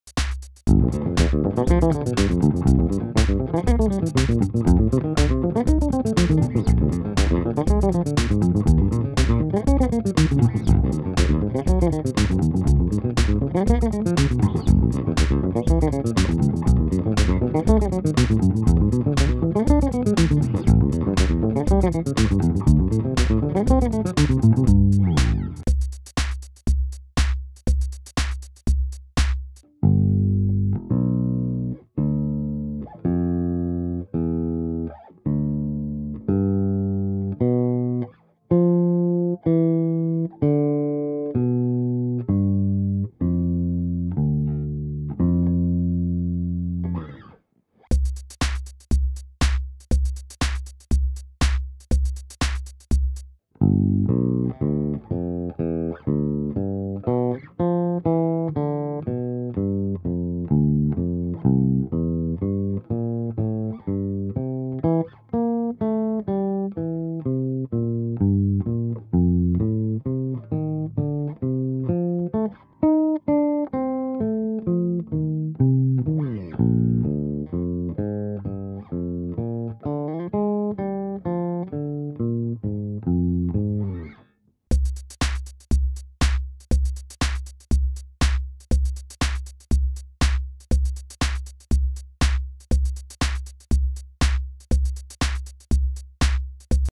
FUN Bass warm up practice - Circle of fourths
This is a fun bass warm-up exercise that I came up with lately.
It's good for practicing the circle of fourth with 7 chords arpeggios.
This exercise also helps if you are trying to memorize the locations of the notes on the fretboard.

Enjoy, and may the funk be with you,
Idogo